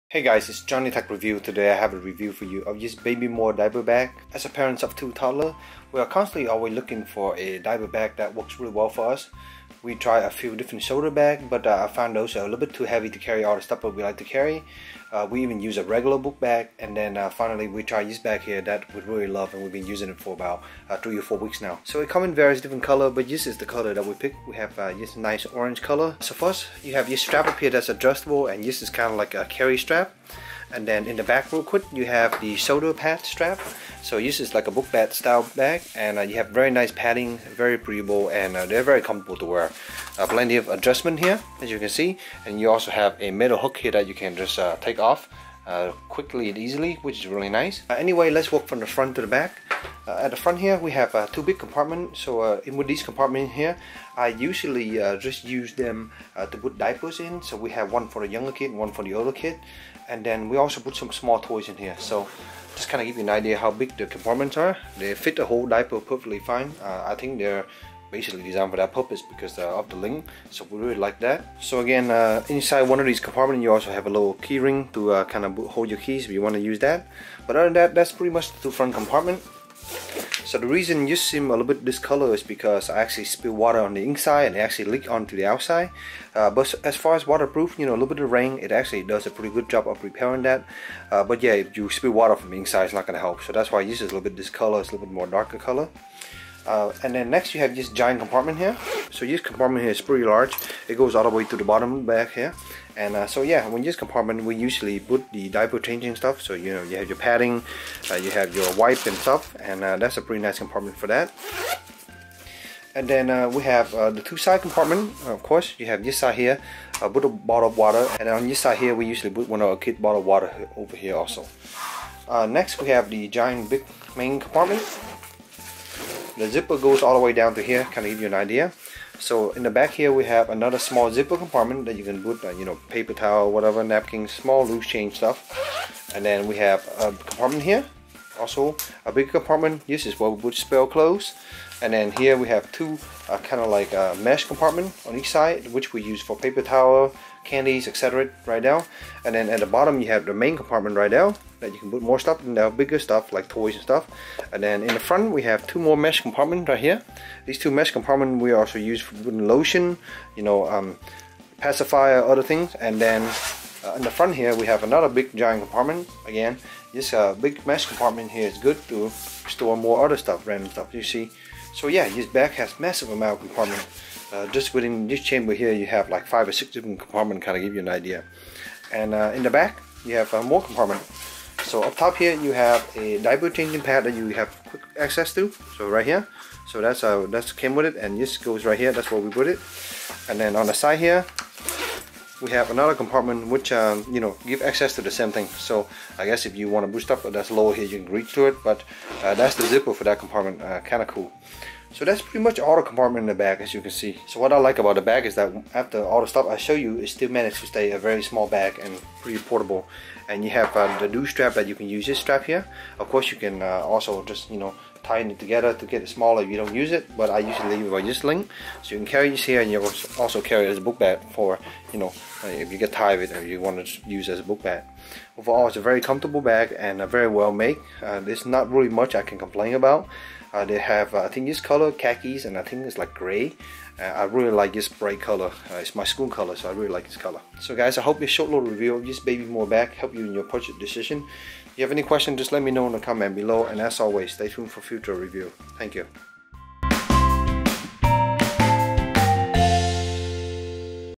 Bebamour Travel Backpack Diaper Bag REVIEW
We have this bag for about a month.  The style of the bag is a hipster type of book bag with loads of compartment and mesh storage spot. Easily carry 2 kid worth of stuff from diapers to exchange set of clothes, to candy, toys, book etc… The comfortable padding makes it easy to wear it when we're out shopping or just strolling about. There's also a warm compartment that's line with heated pad material to keep bottle warm, but we mostly use this to hold the little one water bottle. The two side bottle holder is perfect for holding a regular bottle of water and another smaller bottle for our older toddler. Not much to complain about.